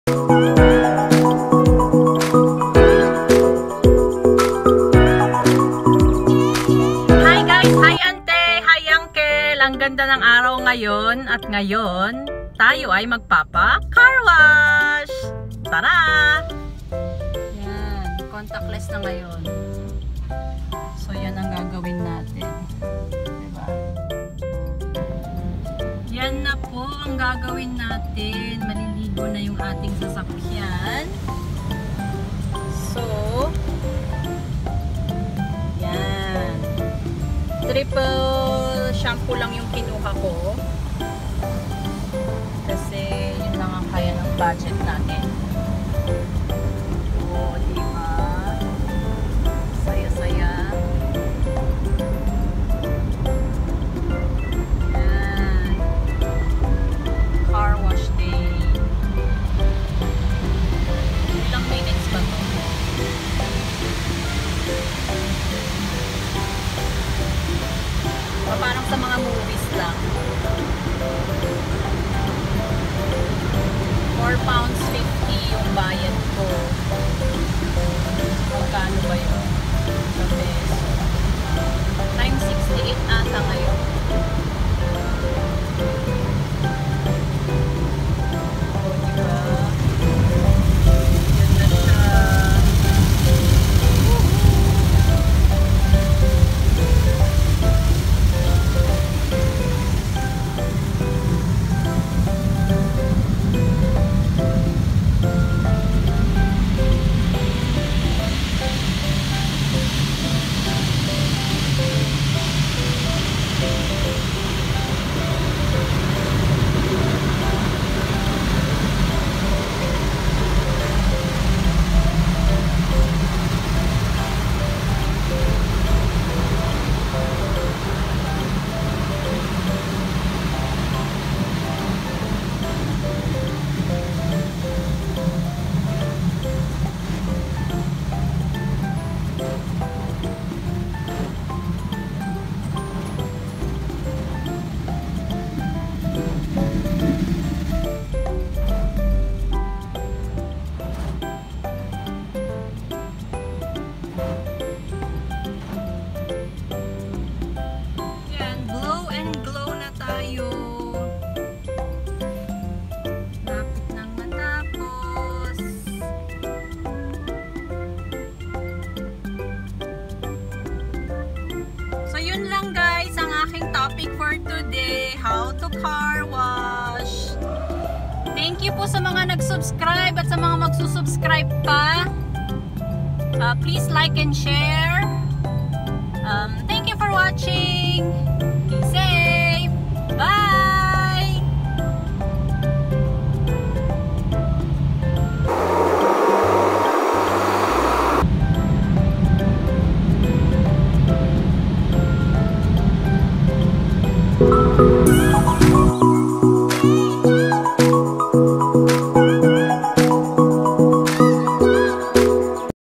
CAREGIVER SA UK LIFE: MAGPACAR WASH PO TAYO 🚘😃
Hi Guys, dahil maaraw ngayon at madumi na rin ang sasakyan tayo ay magpacar wash! Tara samahan nyo ako.